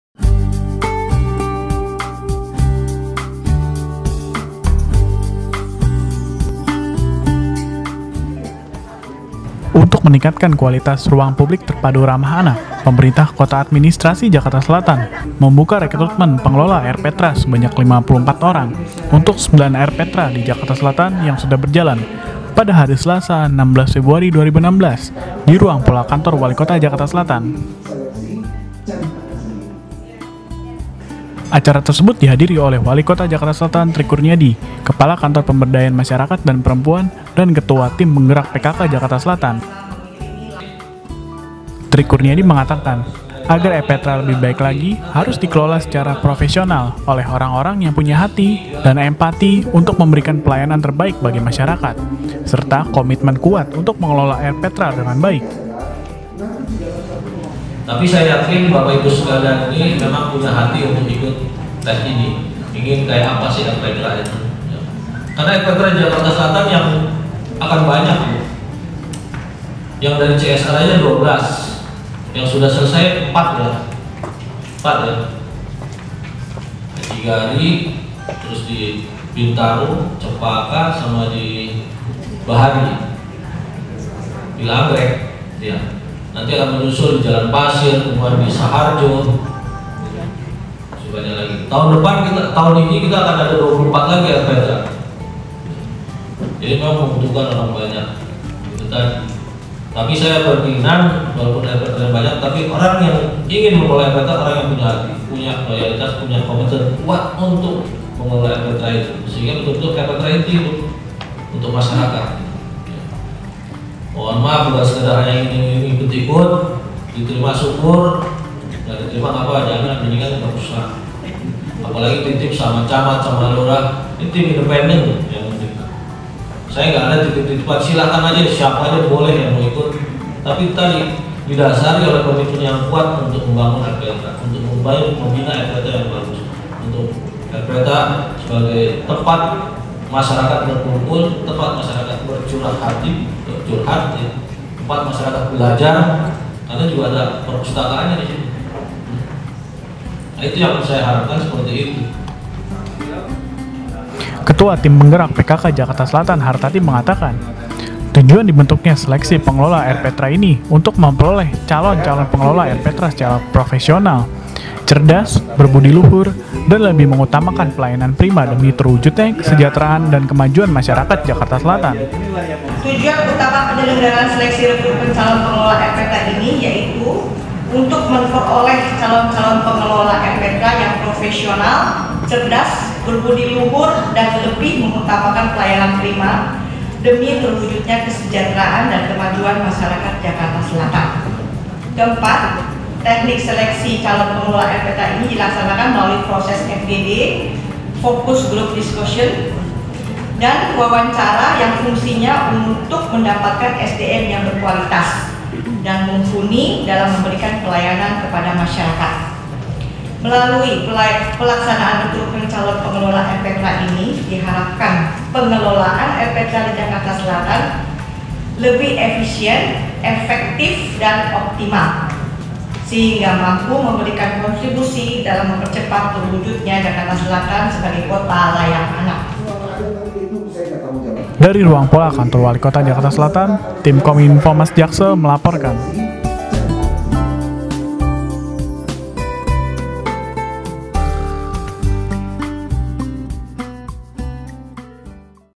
Pembukaan Rekrutmen Pengelola RPTRA Kota Adm. Jakarta Selatan - Selasa 16 Februari 2016
Untuk meningkatkan kualitas Ruang Publik Terpadu Ramah Anak , Pemerintah Kota Adm Jakarta Selatan membuka Rekrutment pengelola RPTRA  sebanyak 54 orang untuk 9 RPTRA di Jakarta Selatan yang sudah berjalan pada hari Selasa 16 Februari 2016,di Ruang Pola Kantor Walikota Jakarta Selatan.